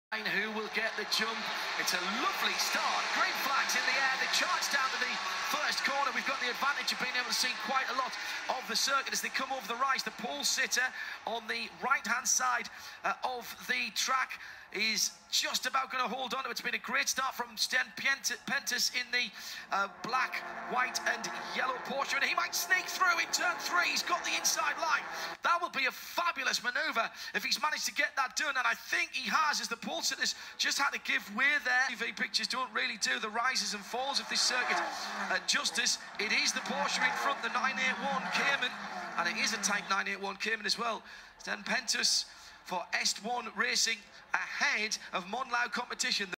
24h Barcelona 2017 race start with Sten Pentus
24h Series Barcelona round 1-3 of September 2017
Est1 Racing team
Porsche Cayman GT4, NR 981
Race start by Sten Pentus